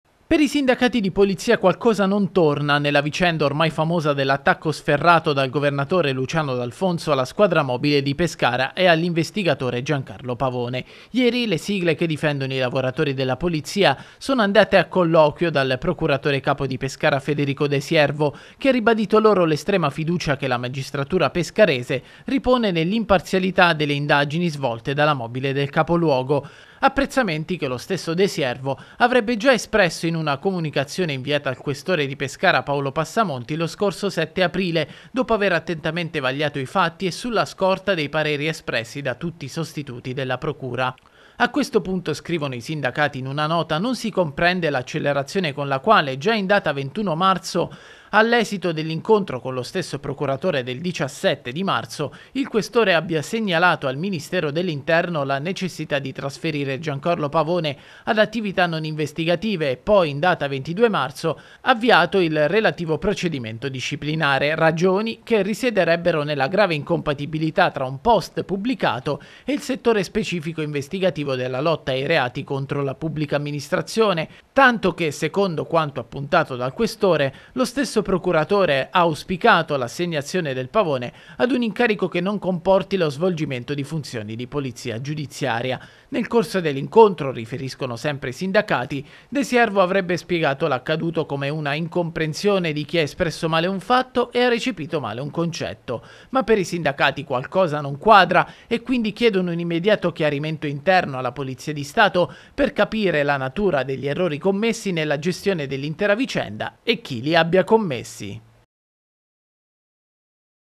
Caso Pavone. Scontro Procuratore-Questore
Il caso della richiesta di trasferimento dell’investigatore della Squadra Mobile di Pescara, Giancarlo Pavone, a seguito degli strali a lui indirizzati dal presidente della Regione Luciano D’Alfonso a seguito delle indagini su La City, si sta intrecciando sempre di più. A colloquio con i sindacati il Procuratore Capo di Pescara Federico De Siervo ha ribadito fiducia alla Polizia pescarese e ha parlato di un malinteso con il Questore Paolo Passamonti